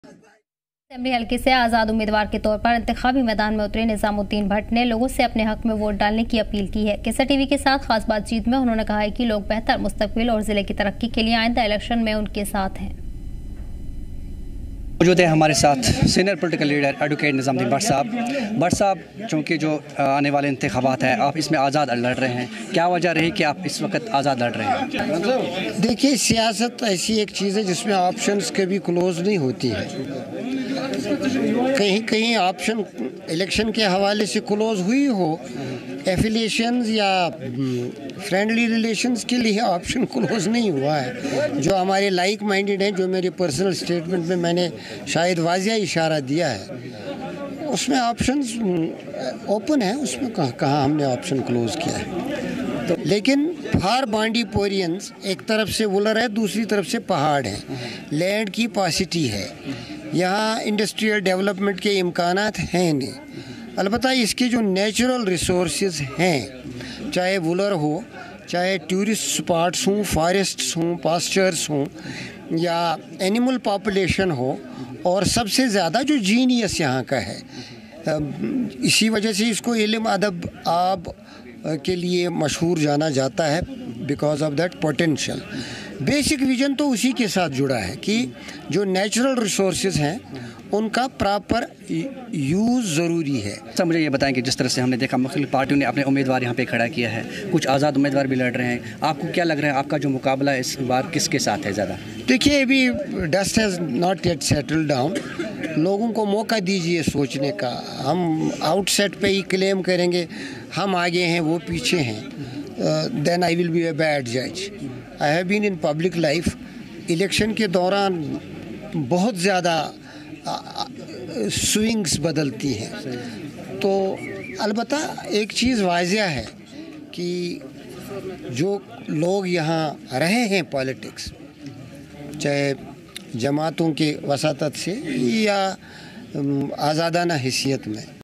Exclusive Interview: Nizamuddin Bhat Shares His Vision for J&K
Jammu and Kashmir Assembly Elections: Independent candidate Nizamuddin Bhat shares his vision and manifesto with Kesar TV in an exclusive interview ahead of first phase voting. Watch to know more!